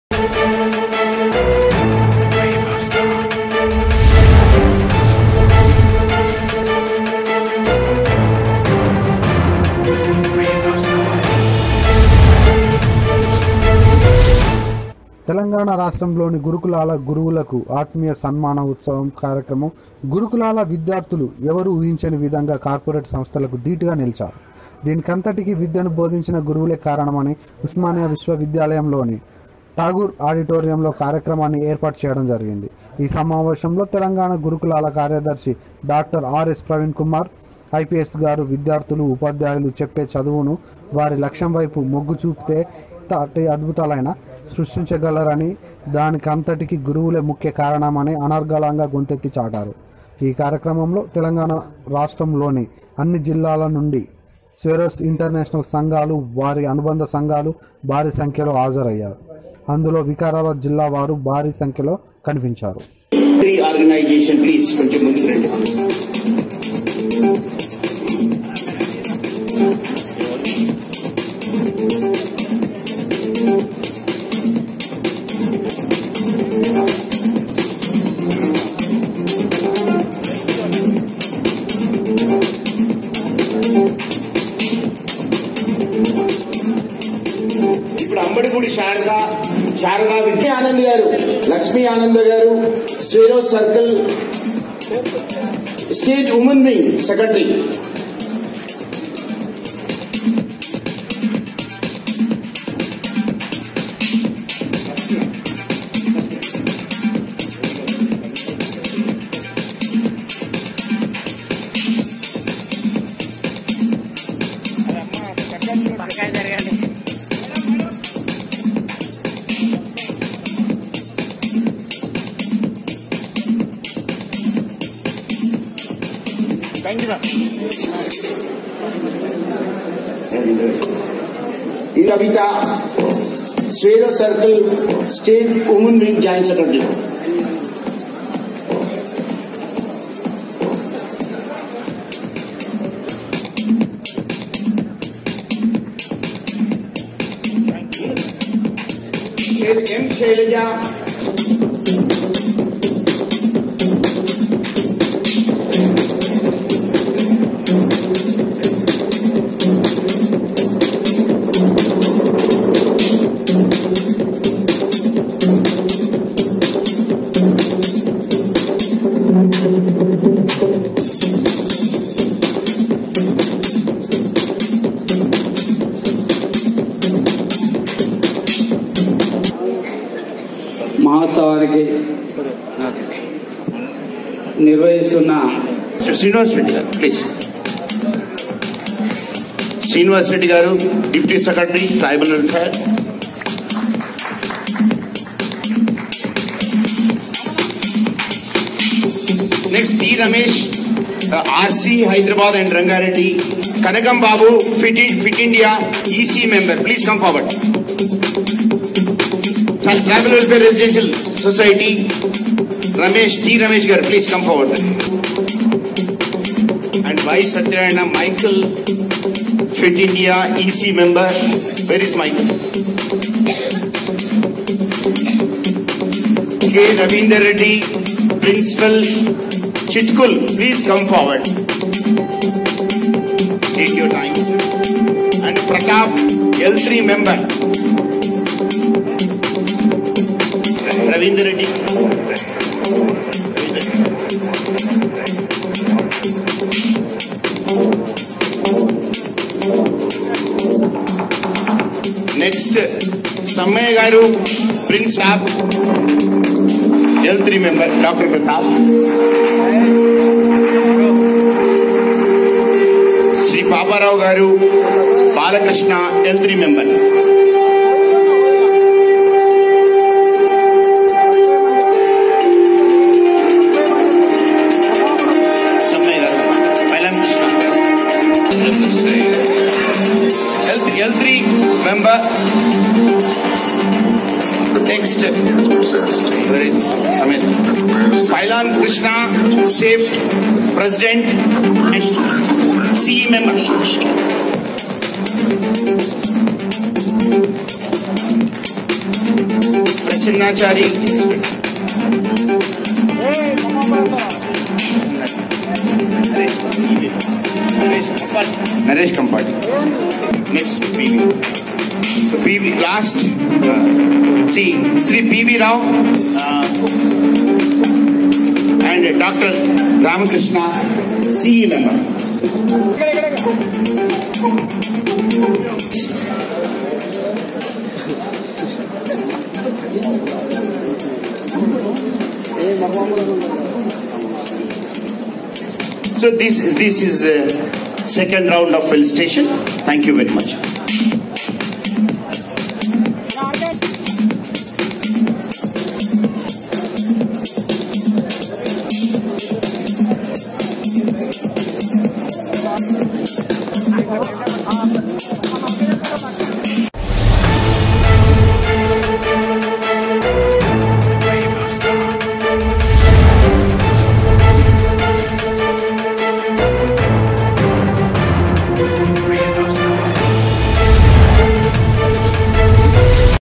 తెలంగాణ రాష్ట్రములోని గురుకులాల గురువులకు ఆత్మీయ సన్మాన మహోత్సవ కార్యక్రమం
తెలంగాణ రాష్ట్రములోని గురుకులాల గురువులకు ఆత్మీయ సన్మాన మహోత్సవ కార్యక్రమం గురుకులాల విద్యార్థులు ఎవరూ ఊహించని విధంగా, కార్పోరేట్ సంస్థలకు ధీటుగా నిలిచారు, దీనికంతటికీ కారణం వారికి విద్యను బోధించిన గురువులే కారణమని ఉస్మానియా విశ్వవిద్యాలయంలోని ఠాగూర్ ఆడిటోరియంలో కార్యక్రమాన్ని ఏర్పాటు చేయడం జరిగింది. ఈ సమావేశంలో తెలంగాణ గురుకులాల కార్యదర్శి డా.ఆర్.ఎస్ ప్రవీణ్ కుమార్ IPS గారు విద్యార్థులు ఉపాద్యాయులు చెప్పె చదువును వారి లక్ష్యం వైపు మొగ్గు చూపుతే ఎంతటి అద్భుతాలనైనా సృష్టించగలరని, దానికంతటికి గురువులే ముఖ్య కారకులని అనర్గళంగా గొంతెత్తి చాటారు. ఈ కార్యక్రమంలో తెలంగాణ రాష్ట్రములోని అన్ని జిల్లాల నుండి స్వేరోస్ ఇంటర్నేషనల్ సంఘాలు, వాటి అనుబంధ సంఘాలు భారీ సంఖ్యలో హాజరయ్యారు. అందులో వికారాబాద్ జిల్లా వారు భారీ సంఖ్యలో కనిపించారు.||N24 NEWS